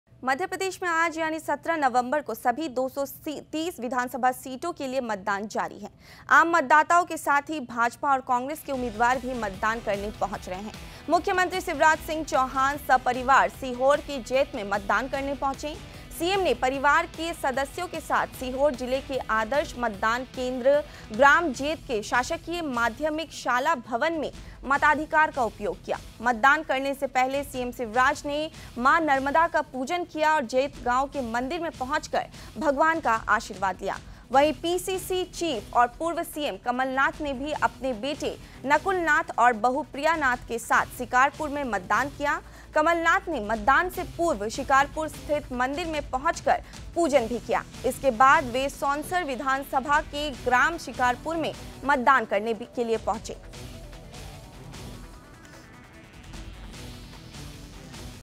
विधानसभा सीटों के लिए मतदान जारी…
विधानसभा सीटों के लिए मतदान जारी….भाजपा और कांग्रेस के उम्मीदवार भी मतदान करने पहुंचे…मतदान करने से पहले सीएम शिवराज ने मां नर्मदा का पूजन किया….पूर्व सीएम कमलनाथ ने भी शिकारपुर स्थित मंदिर में किया पूजन ……